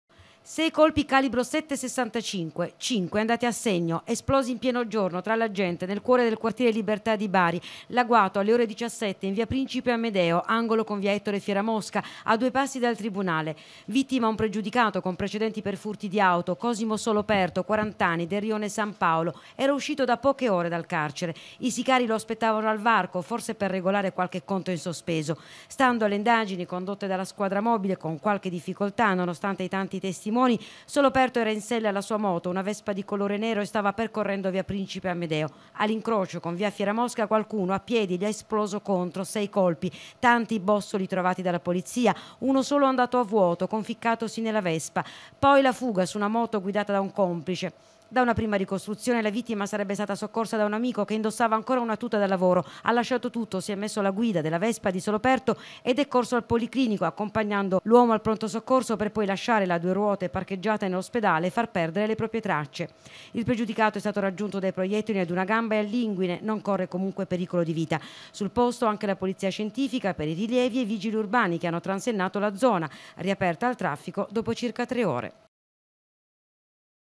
BARI AGGUATO AL QUARTIERE LIBERTA'
Sei colpi calibro 7, 65 5 andati a segno esplosi in pieno giorno tra la gente nel
cuore del quartiere liberta di bari l'agguato alle ore 17 in via principe
amedeo angolo con via ettore fieramosca a due passi dal tribunale, vittima un
pregiudicato con precedenti per furti di auto cosimo soloperto 40 anni del rione
san paolo, era uscito da poche ore dal carcere,  i sicari lo aspettavo al varco
forse per regolare qualche conto in sospeso stando alle indagini condotte
dalla squadra mobile con qualche difficoltà nonostante i tanti testimoni
Soloperto era in sella alla sua moto, una vespa di colore nero e stava percorrendo
via principe amedeo, all'incrocio con via fieramosca qualcuno a piedi  gli ha esploso
contro sei colpi, tanti i bossoli ritrovati dalla polizia uno solo andato a vuoto
conficcatosi nella vespa, poi la fuga sulla moto guidata un complice. Da una
prima ricostruzione la vittima sarebbe stata soccorsa da un amico che indossava
ancora una tuta da lavoro,  ha lasciato tutto, si è messo alla guida della vespa
di Soloperto ed è corso al policlinico accompagnando l'uomo al pronto soccorso
per puoi lasciare la due ruote parcheggiata in ospedale far perdere le
proprie tracce
il pregiudicato è stato raggiunto dai proiettili ed una gamba e all'inguine, non
core comunque pericolo di vita sul posto anche la polizia scientifica per i
rilievi, i vigili urbani che hanno transennato la zona riaperta al traffico
dopo circa tre ore